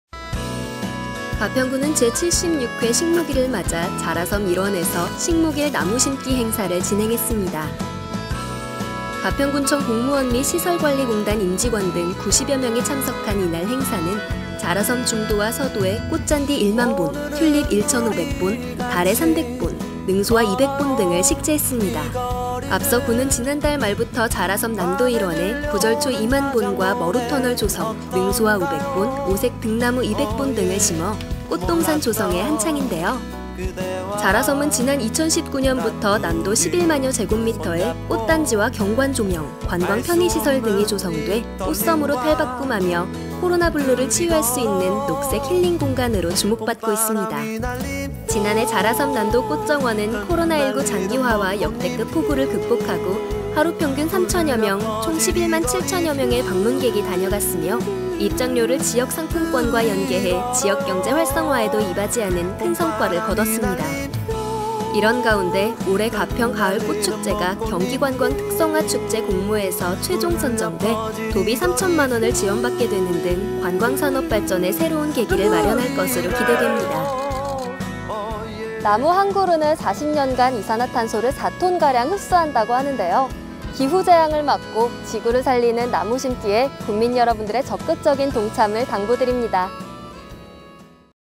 가평군, 제76회 식목일 맞아 자라섬에서 나무심기 진행
가평군은 제76회 식목일을 맞아 자라섬 일원에서 가평군청 공무원 및 시설관리공단 임직원 등 90여명이 참석해, 자라섬 중도와 서도에 꽃잔디 1만본, 튤립 1천500본, 다래 300본, 능소화 200본 등을 식재했습니다.